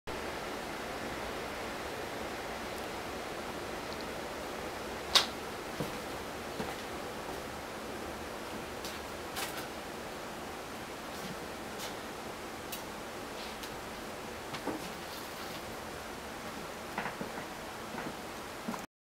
Ken's Bike - Studio 8/11/09
Ken Taking his bike off the stand, in my studio after painting.